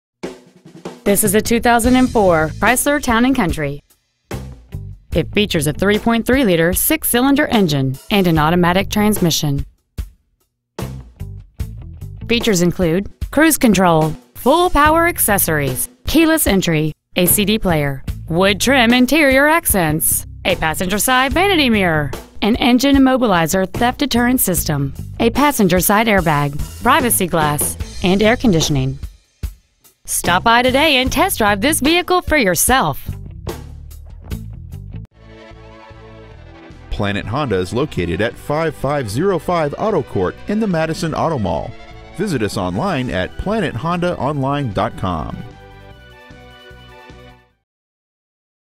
2004 Chrysler Town Country Dyer IL
We've been honored to serve the Matteson IL area , we promise that your experience at our dealership will exceed your expectations !

 Year : 2004
 Make : Chrysler
 Model : Town and Country
 Engine : 3.3L
 Trans . : AUTOMATIC
 Exterior : Midnight Blue Pearl
 Miles : 90,157
 Interior : Med Slate Gray
 Stock : 20642

 5505 Auto Ct.

 Preowned 2004 Chrysler Town and Country Matteson IL